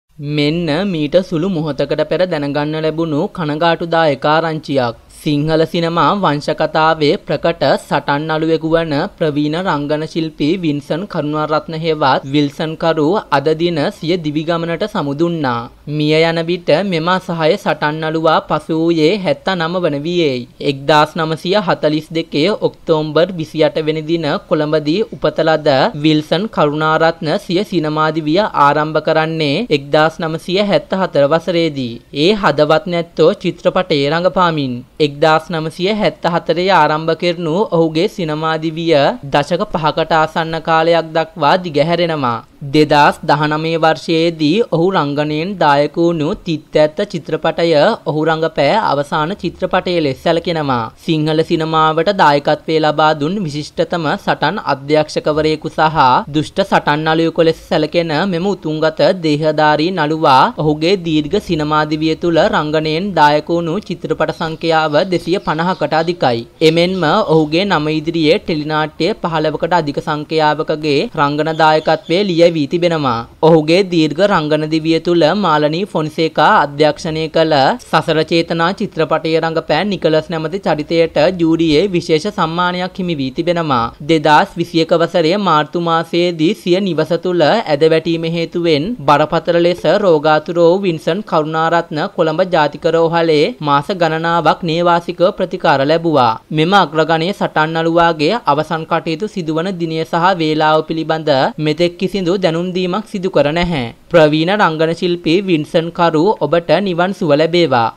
Veteran actor Wilson Karu passes away
Veteran actor Wilson Karu passes away - ප්‍රවීණ රංගන ශිල්පි විල්සන් කරූ මෙලොවින් සමුගනියි